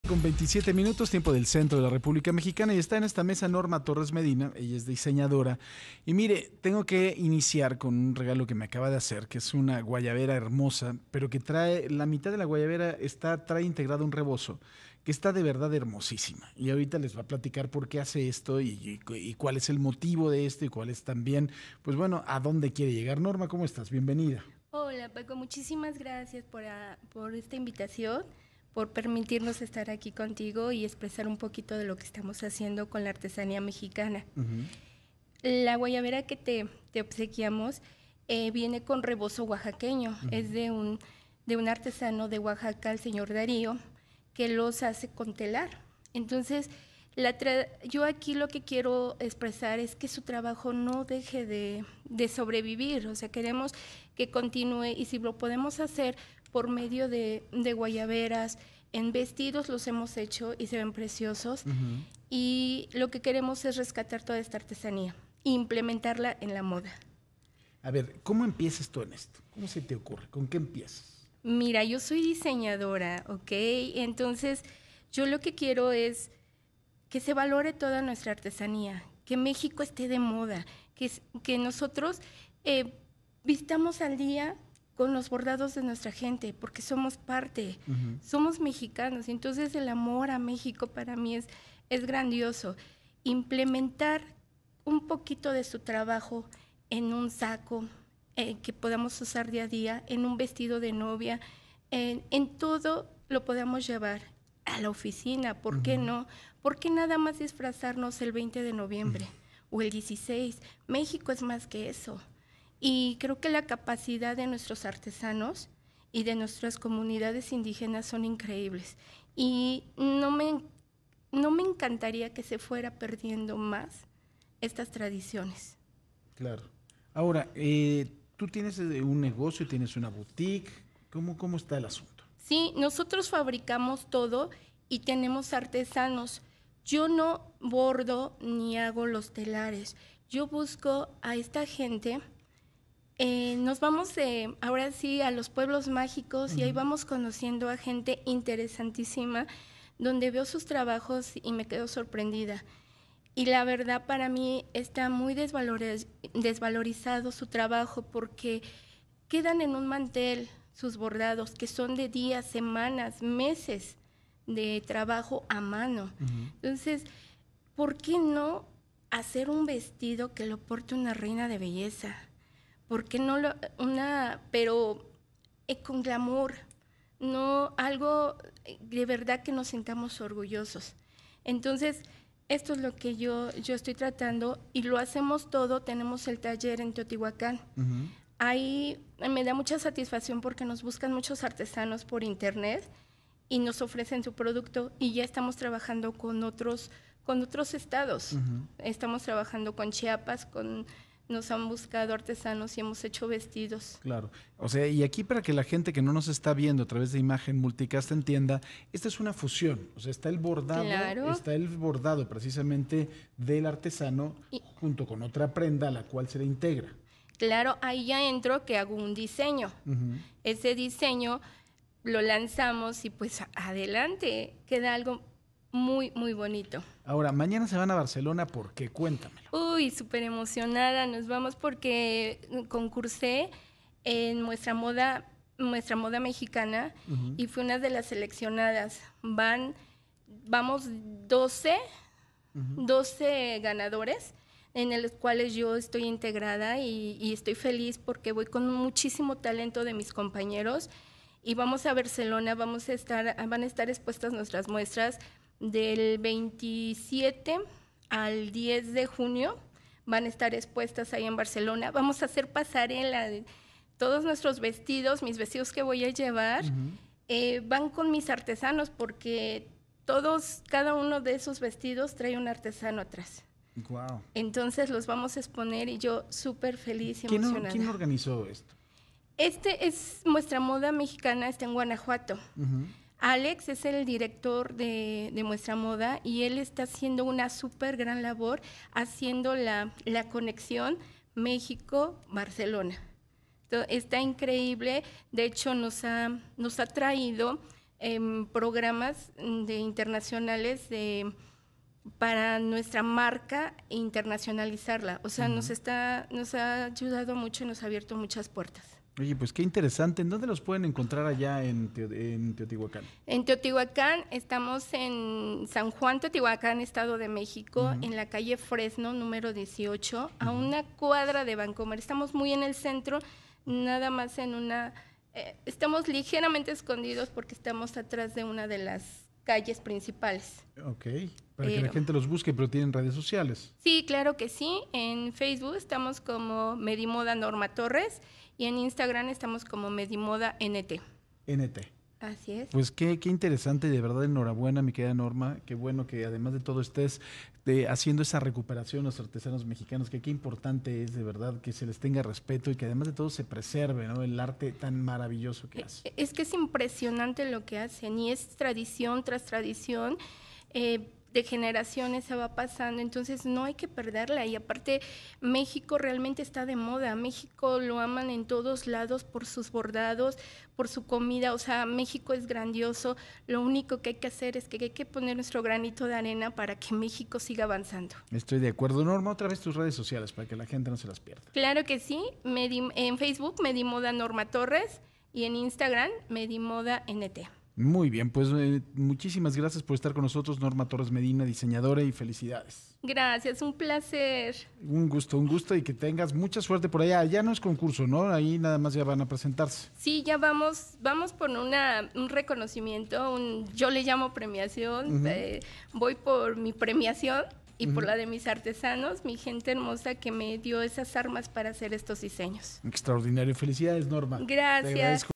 Artesanos mexicanos viajan a Barcelona para exponer sus bordados
Norma Araceli Torres, diseñadora de moda, habla en entrevista sobre la importancia de preservar las artesanías que son características de cada uno de los estados  del país, destacó que trabajan de la mano con los artesanos para ayudarles a crear piezas que se puedan usar todos los días y tener un mejor ingreso económico.